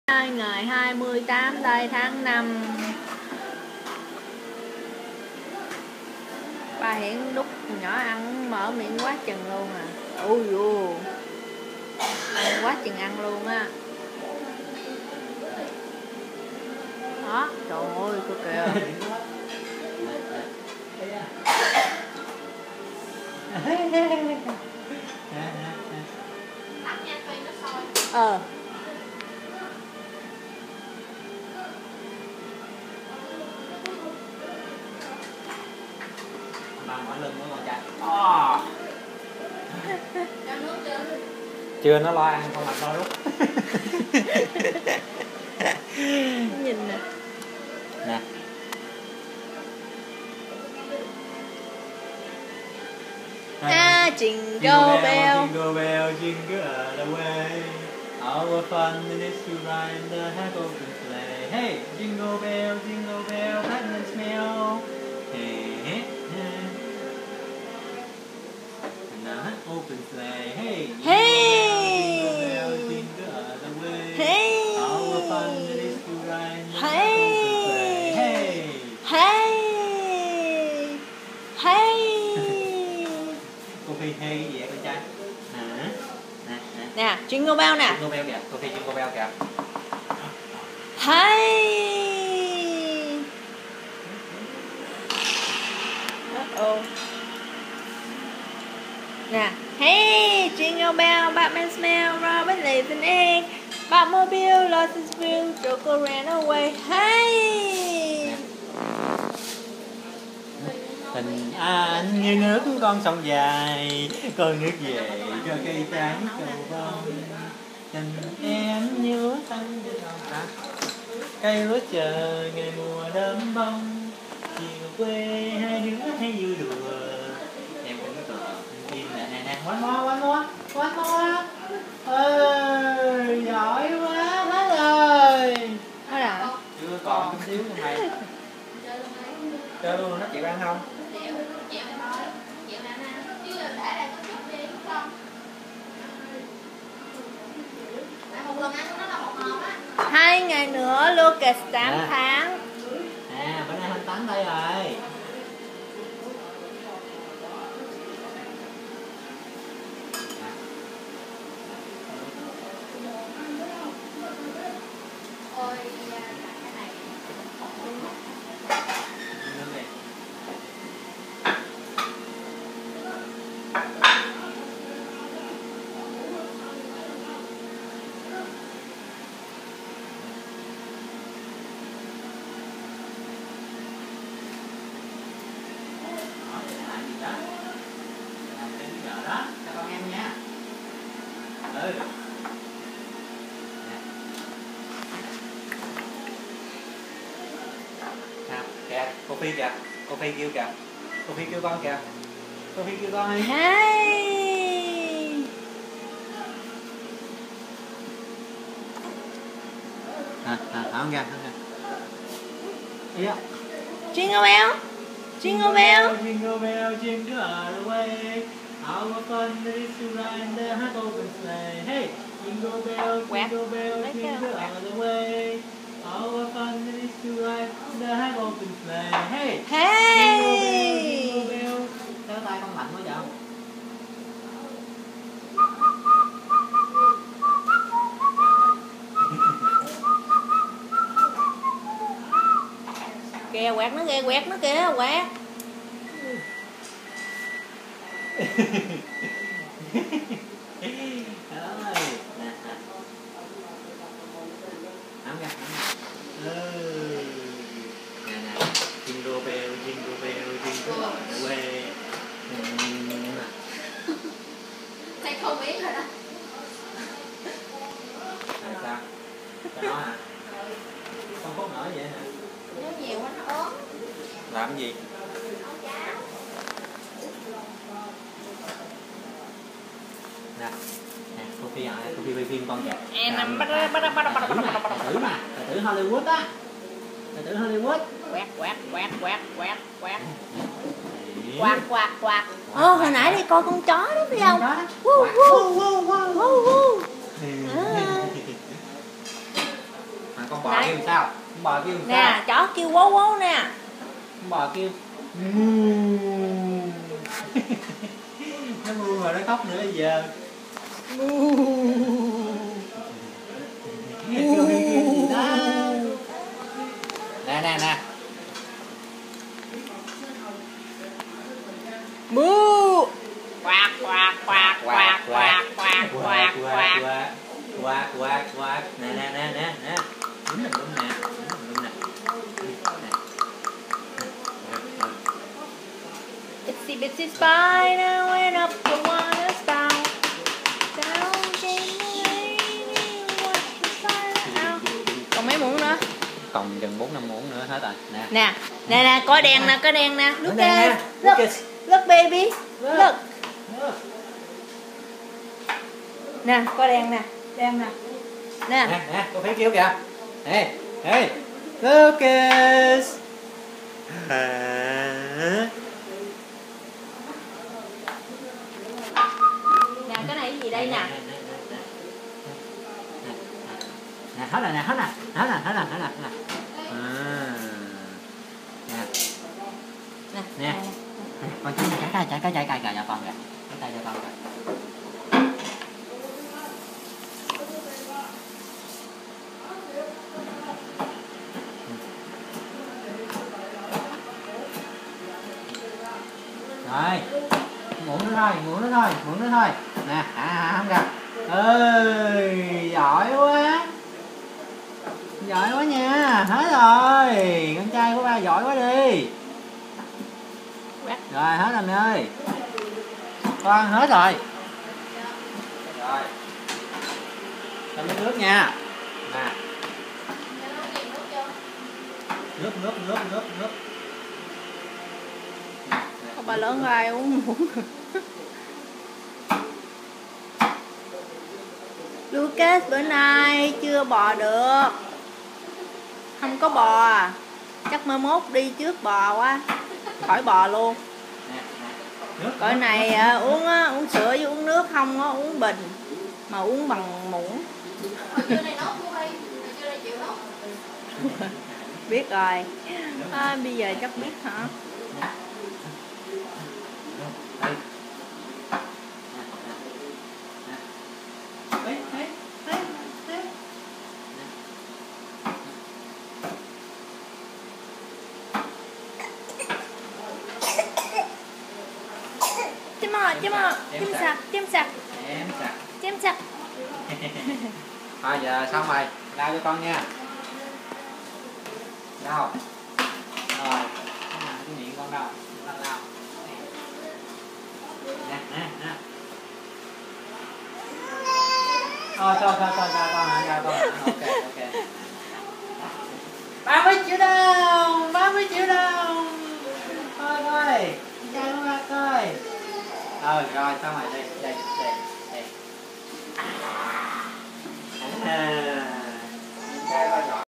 Ba Hien feeding baby Lucas 5/28/14
5/28/14 Two more days Lucas will be 8 months